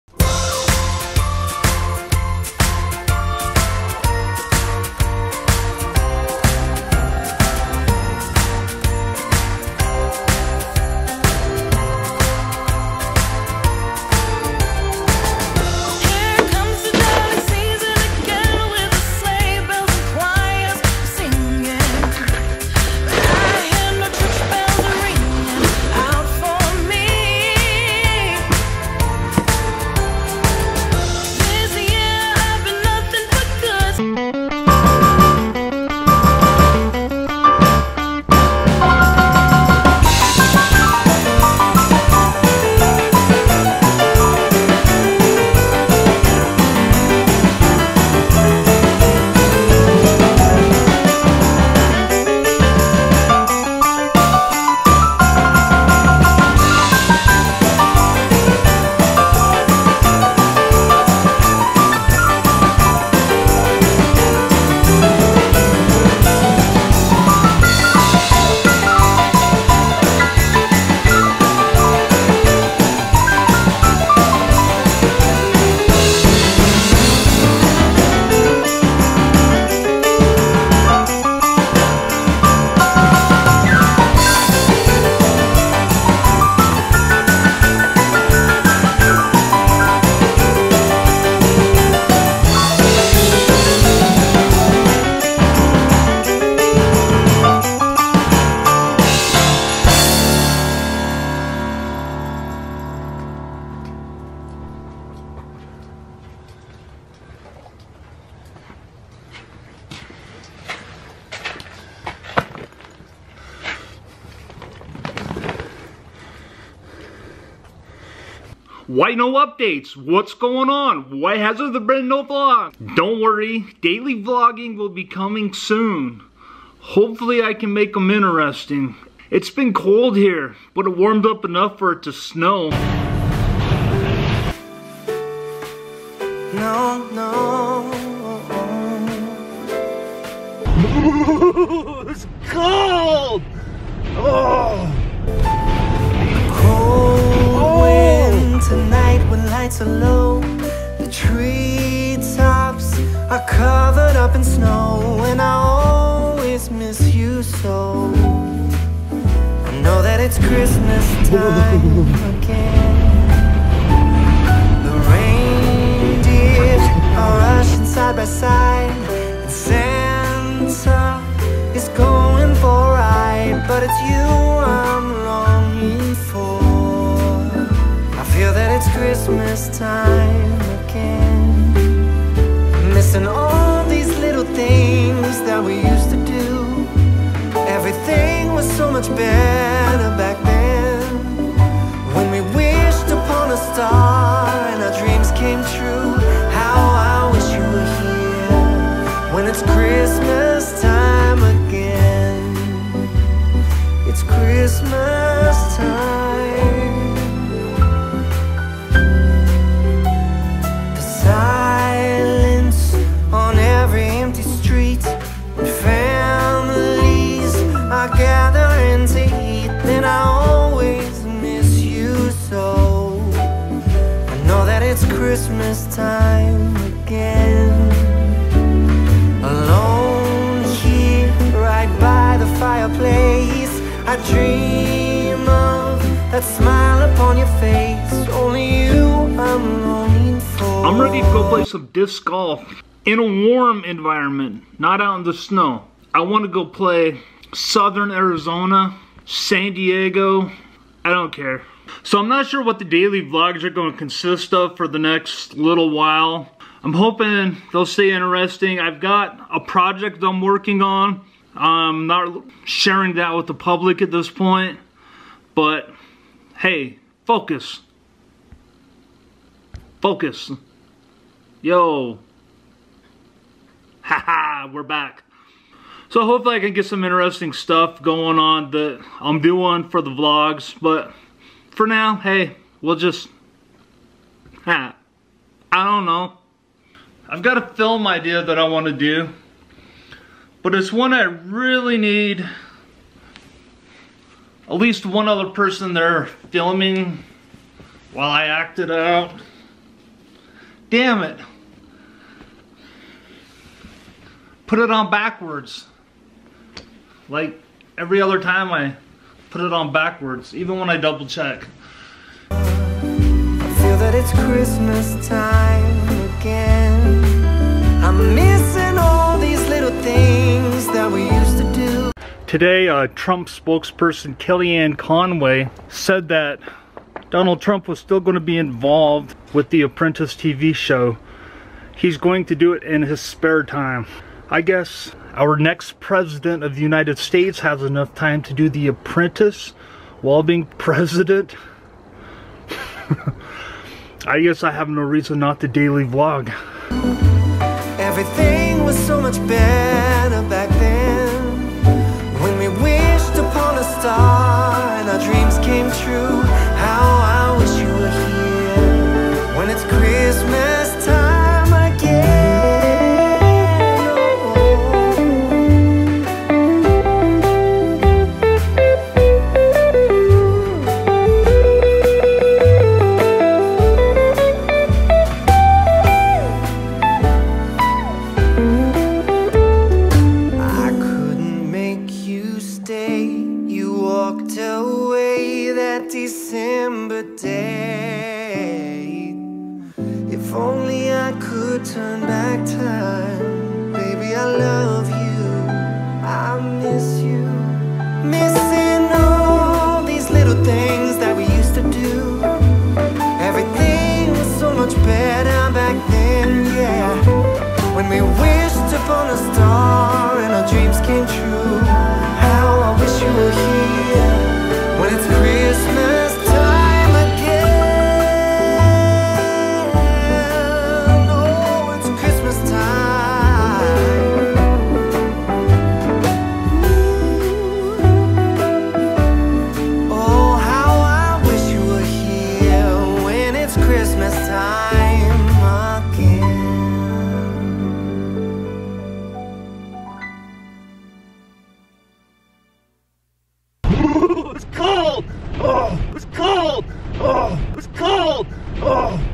It's Snowing in Idaho- Vlog 10
It's snowing in Pocatello and I'm dreaming of a warm disc golf course.